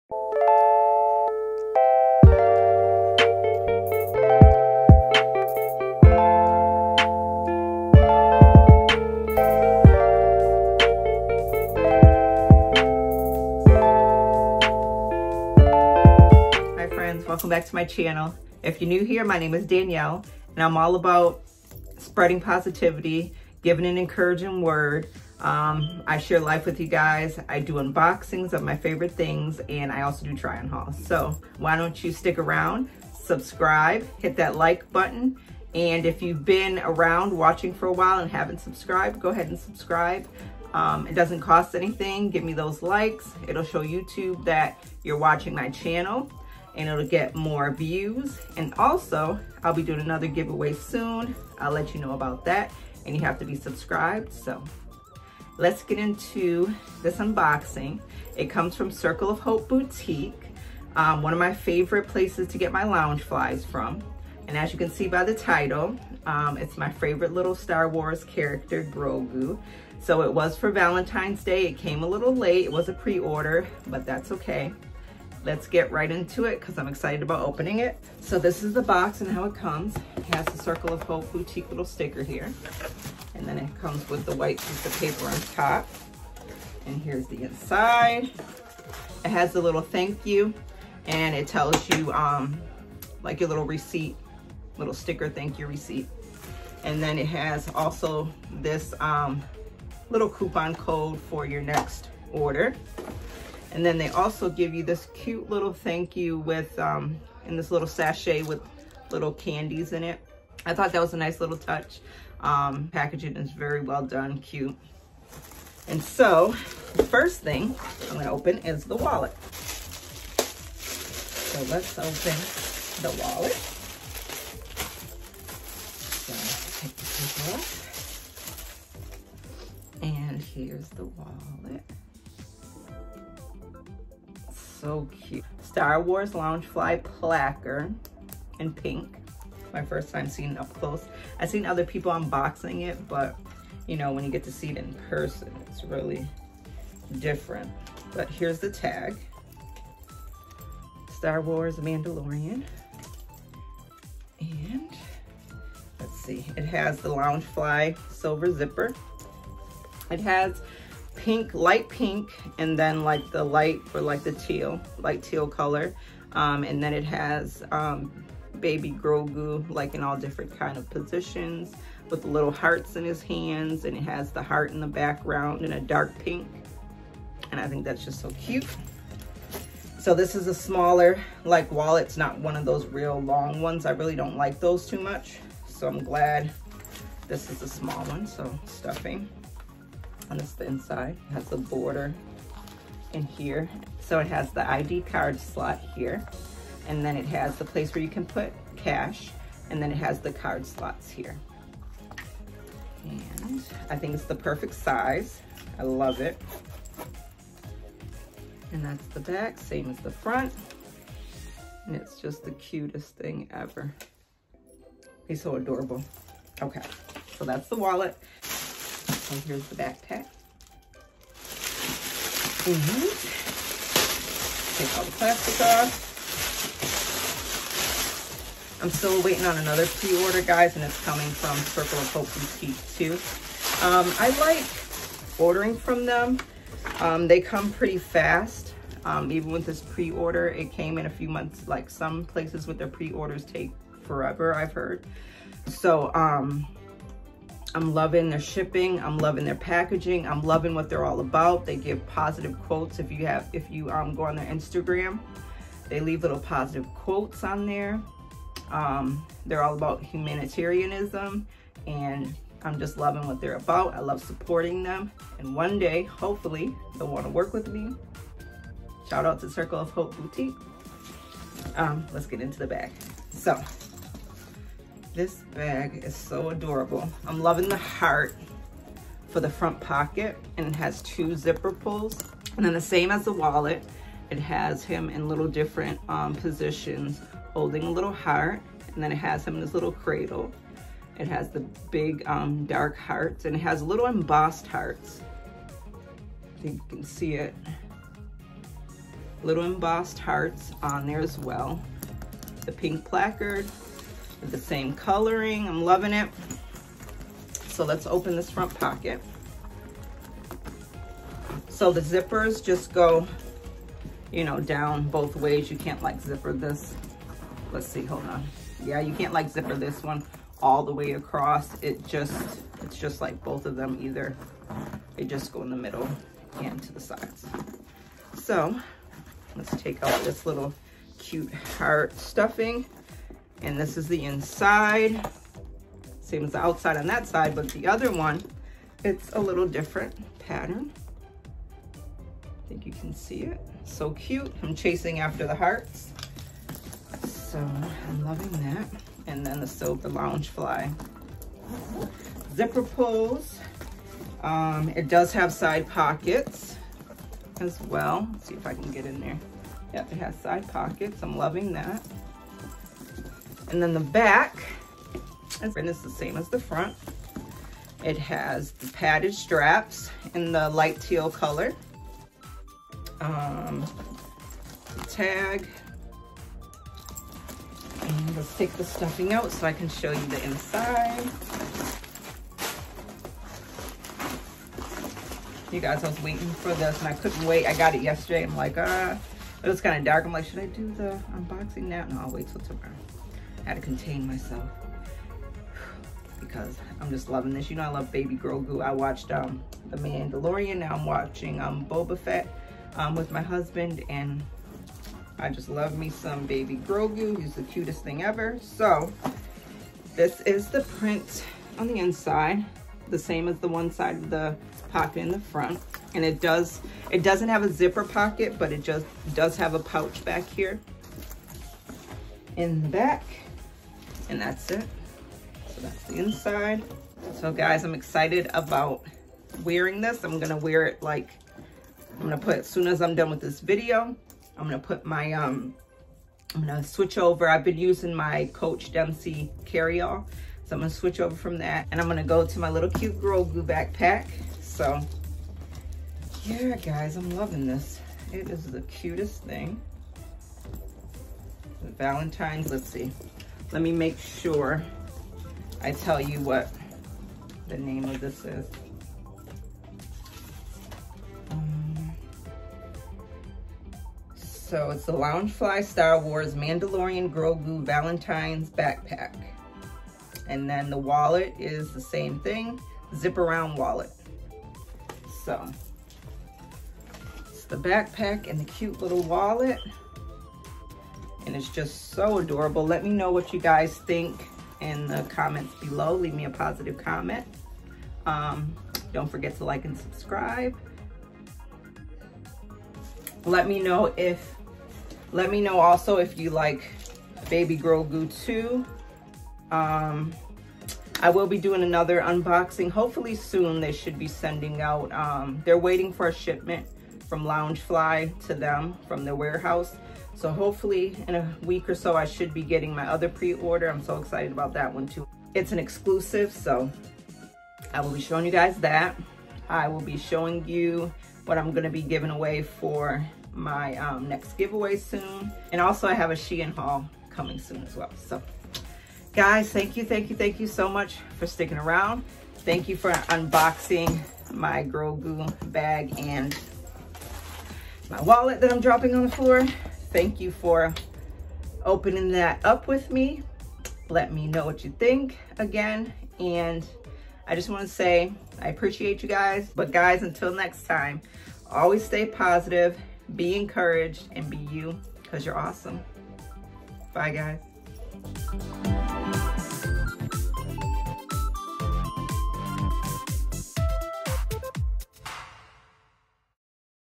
New loungefly unboxing/ 💕 Grogu valentines backpack and wallet💕
Hi friends! So cute 🥰 as I keep saying lol. Let me know what you think. Stay awesome.